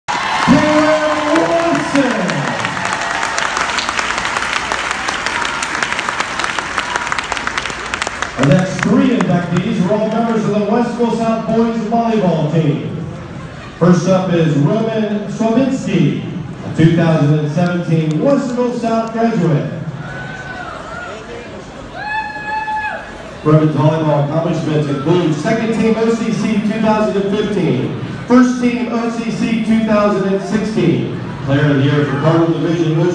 Ohio State’s Kaleb Wesson
Ohio State basketball player and Westerville South graduate Kaleb Wesson was one of eight former Wildcats inducted into the school’s athletics hall of fame Jan. 5. [Frank DiRenna/ThisWeek]